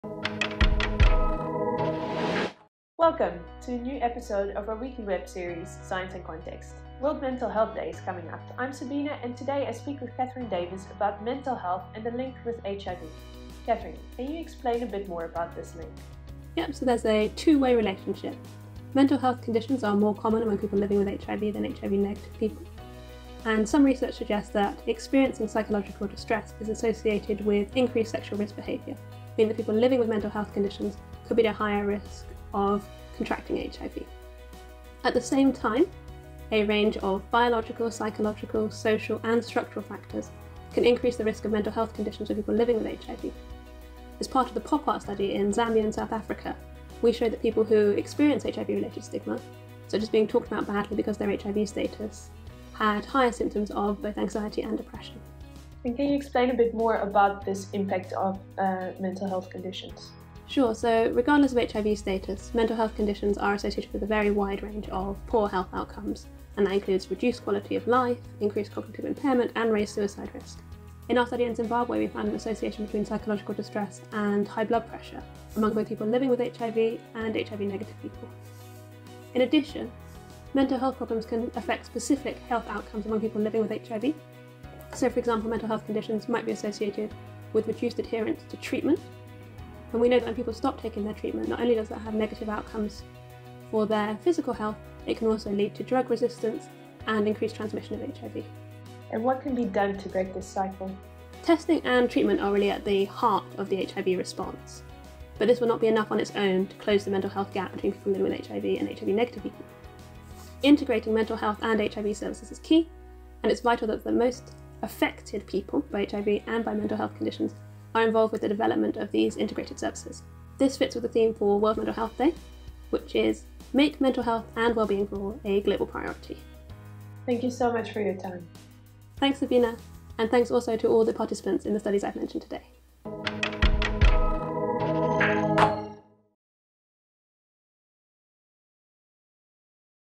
Ep 48 Science in Context - HIV with Katherine Davis (06-10-2022)
In this episode of "Science in Context", Dr Sabine van Elsland speaks with Katherine Davis about HIV and mental health.

Expert: Katherine Davis, Imperial College London
Presenter and Producer: Dr Sabine van Elsland, Imperial College London

If you or someone you know are feeling emotionally distressed or are thinking about suicide, you are not alone.

Contact Samaritans on 116 123 (in UK), or Contact Befrienders Worldwide for support